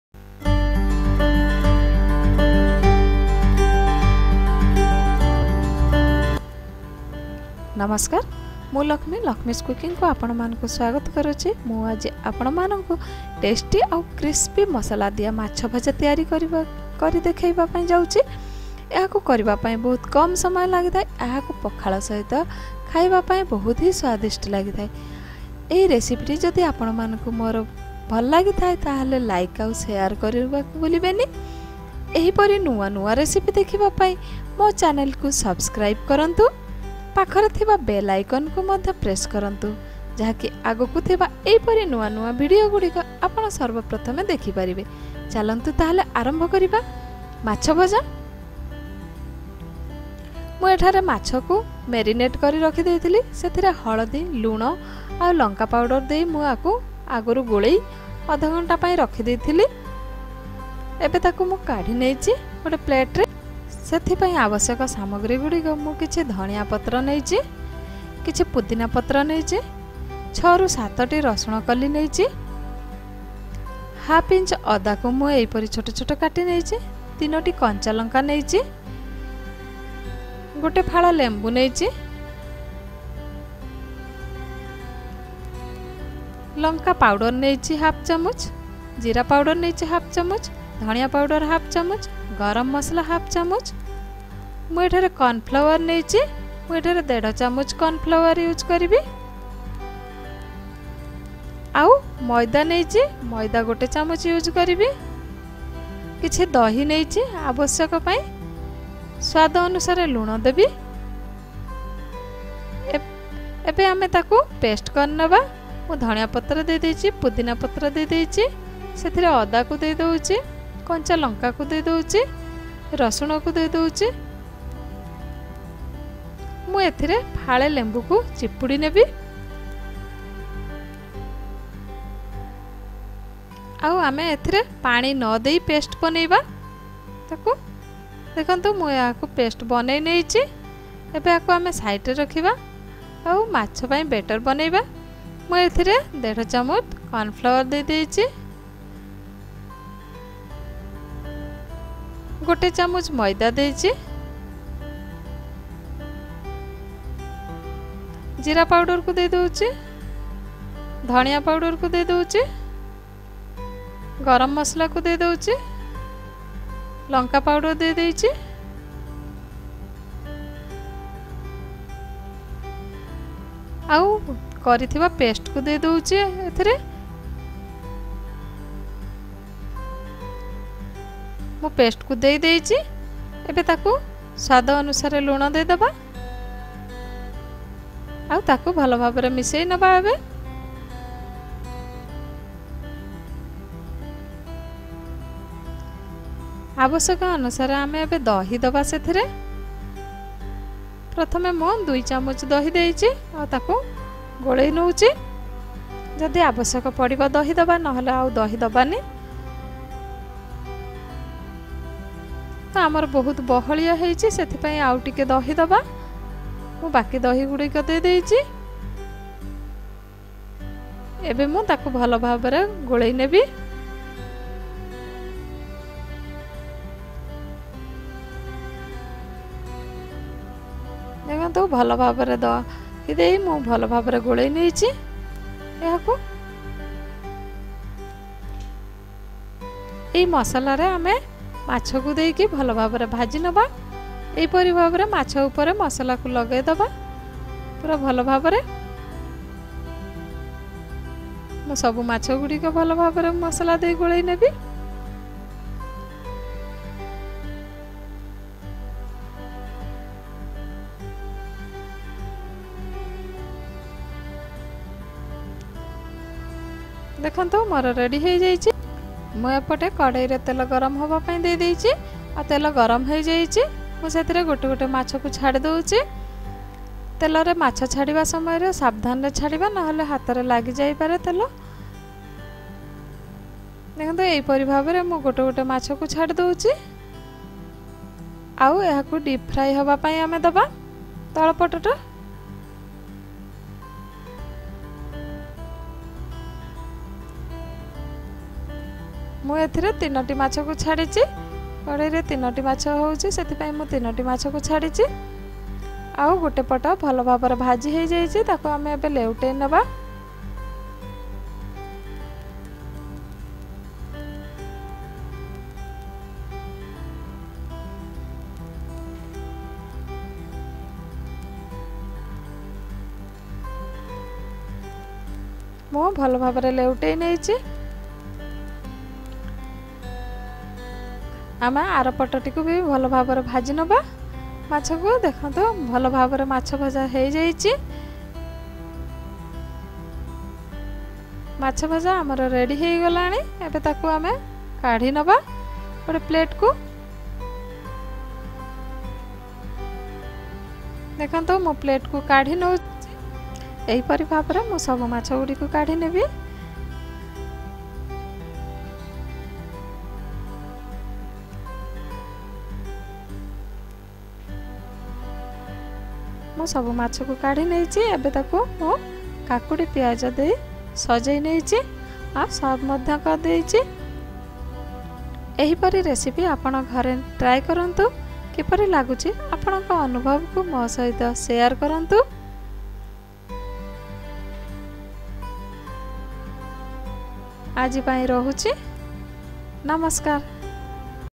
FRY FISH MASALA RECIPE || How to make Crispy Fish Fry recipe || ହୋଟେଲ ପରି ଘରେ ବନ
Fish Masala Fry Testy Recipe \ Crispy Fish Fry recipe \  ହୋଟେଲ ପରି ଘରେ ବନାନ୍ତୁ କ୍ରିସପି ମାଛ ଭଜା

Ingredients For Crispy Fish Fry :-
Fish – 3pic
Corn Flour – 1,1/2 tbsp
Maida – 1 tbsp
Coriander leaves -2,3 tbsp
Mint Leaves (Pudina) -2 tbsp
Lemon -1/2
Cumin powder (jira) -1/2 tbsp
Coriander powder (Dhania) -1/2 tbsp
Salt – test according
Oil – for fish fry